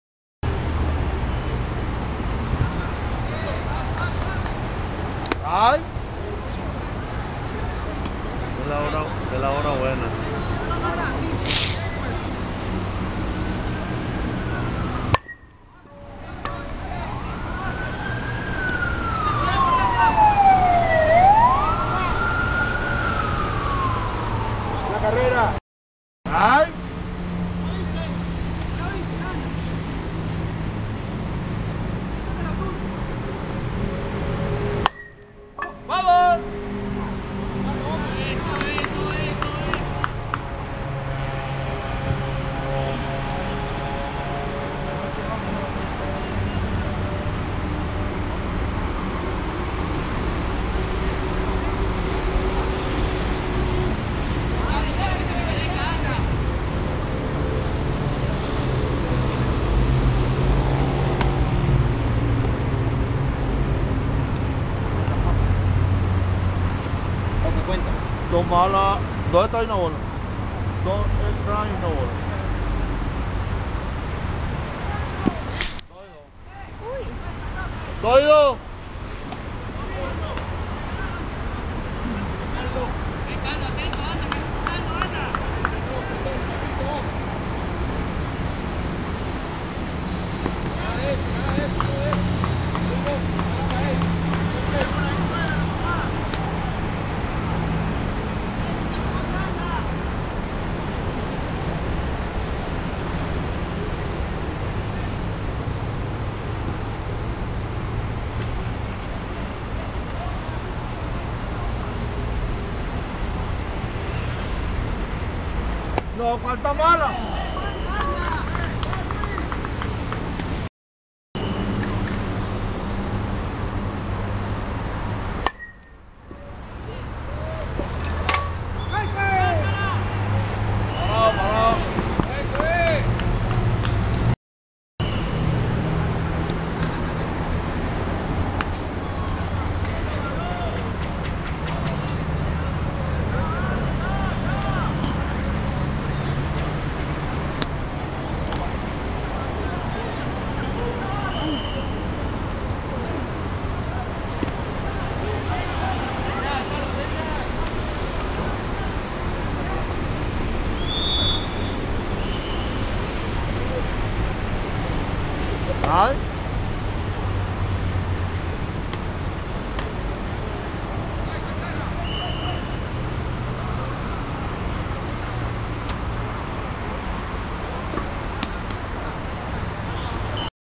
Carlos Chavez frente Mirandino 2/2/2016
8vo juego de la ronda eliminatoria Copa Navidad 2015/2016 Diamantes vs Mirandino